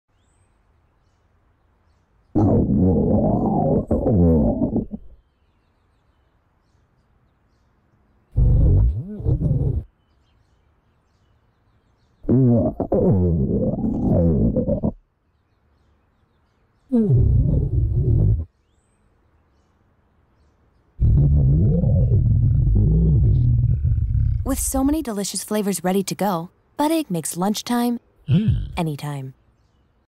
Buddig 'Stomach Growls' Commercial (30 Second Version)
I bet these 2 stomachs can communicate with these 2 guys!